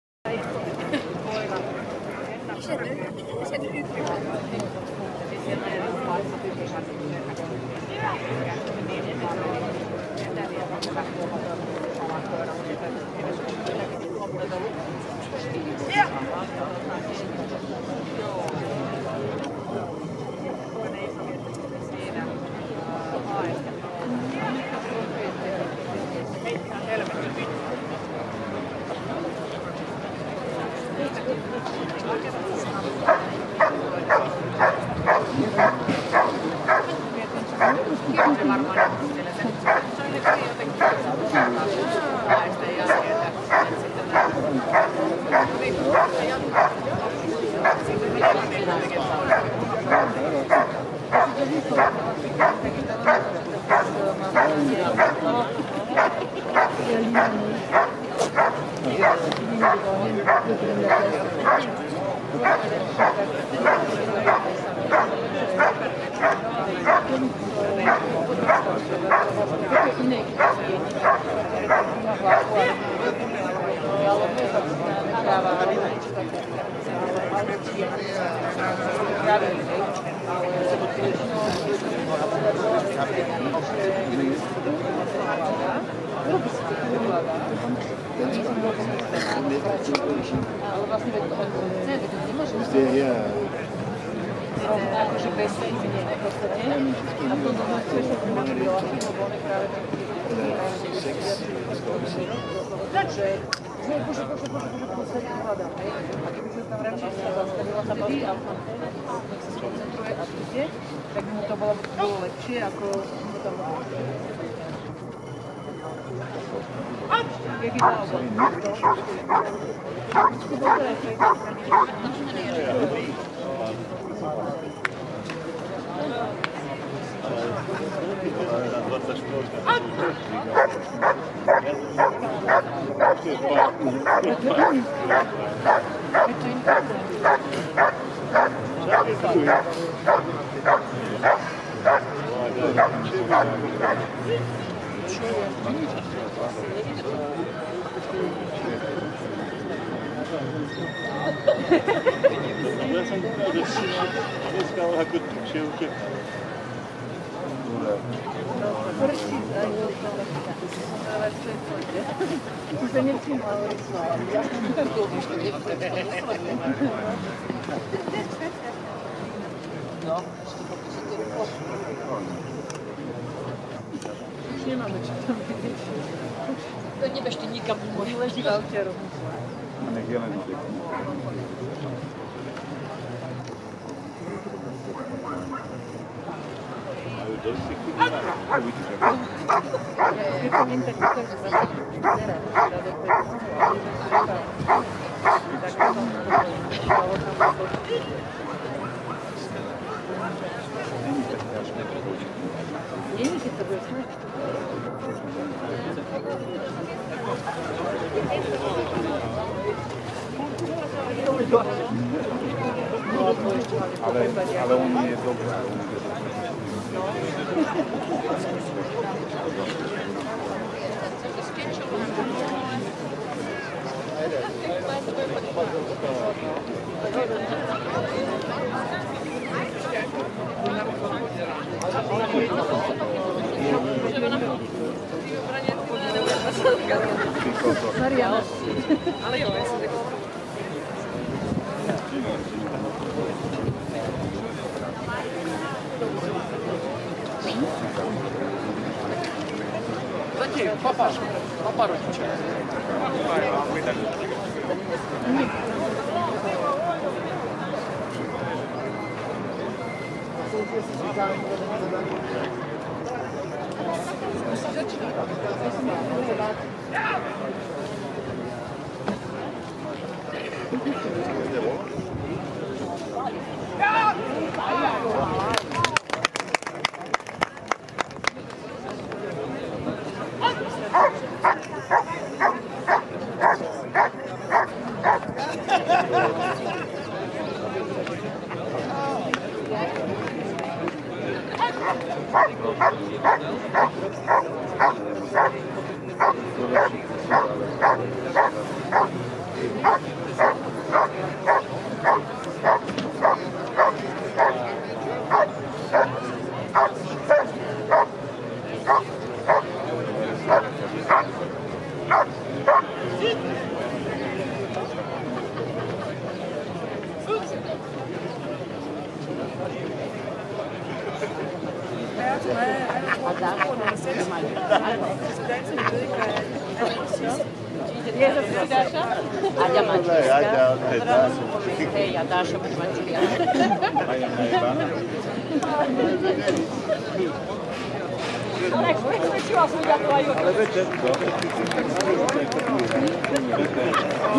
WUSV 2021 CAN 1 - ERIC LEBLANC & PYXX VON NARNIA C 91
WUSV 2021 LERMA- BURGOS- ESPAÑA
1 AL 7 NOVIEMBRE 2021
A:    B:    C:  91
JUECES:
A: HENRIETTE BOHNSTEDT – UK
B: FRANK PHILLIPS – USA
C: HORST DIETER TRÄGER – GERMANY
FIGURANTES:
LUCAS MARTINEZ SANCHO
AARON MARTINEZ SACRISTAN